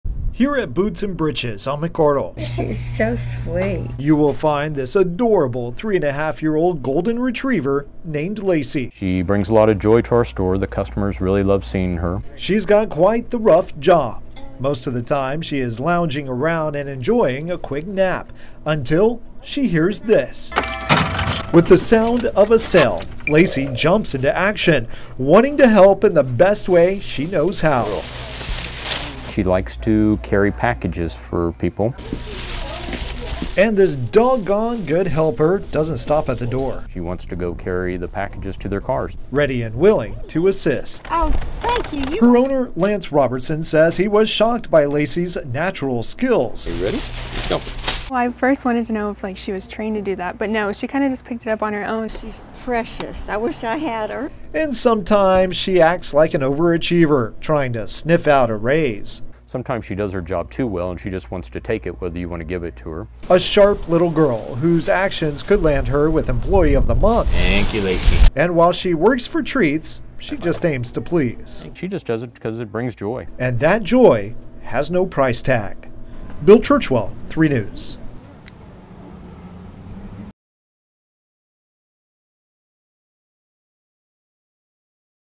This Golden Retriever will carry your bags to your car
As soon as Lacy hears the sound of the cash register, she springs to action.